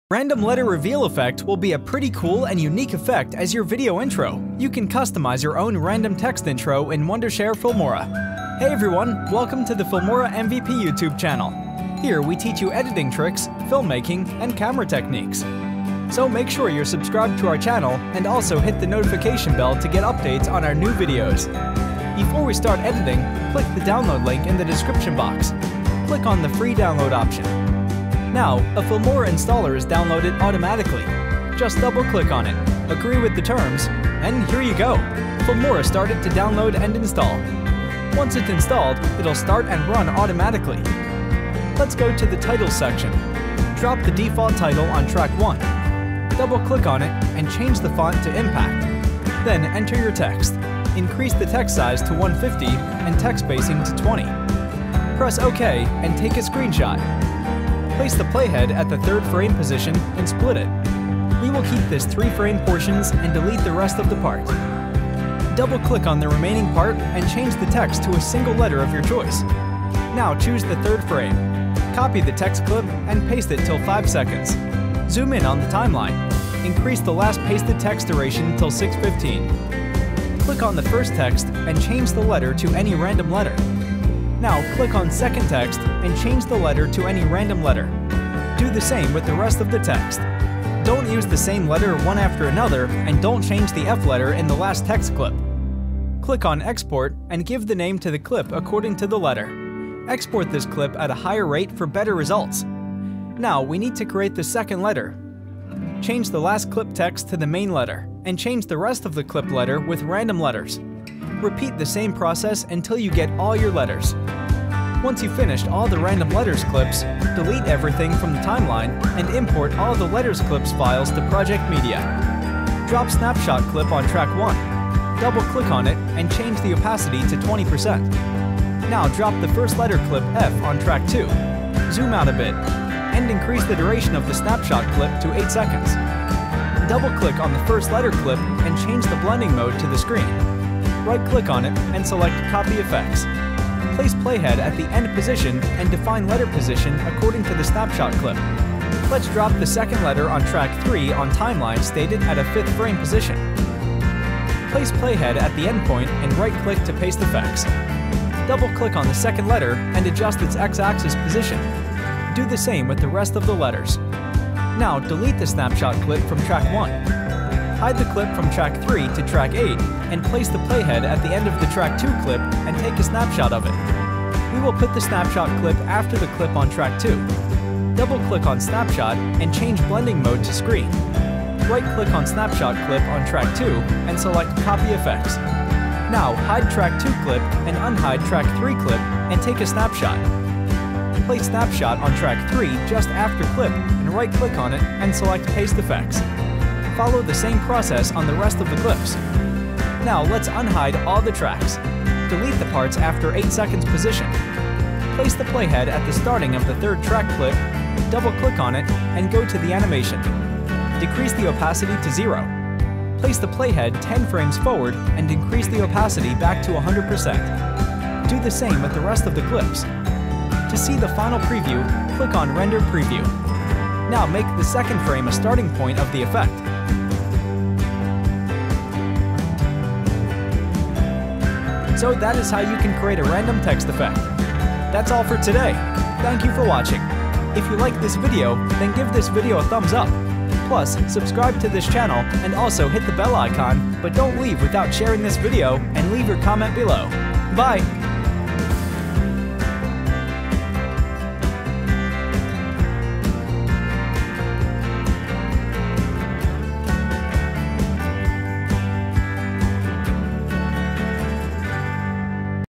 How to Make Random Letter Reveal Intro
In this video, we will teach you how to make random letter reveal intro in Filmora.